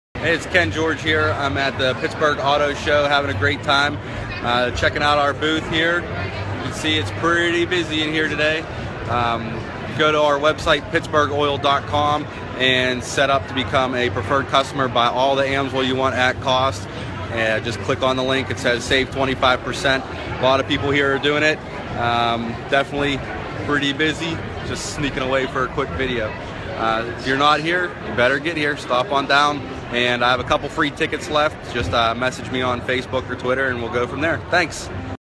Pittsburgh International Auto Show sneak peak of the madness!
A quick view into the Pittsburgh International Auto Show in 2016! We had a blast at the David L Lawrence Convention Center and we can't wait until 2017 show!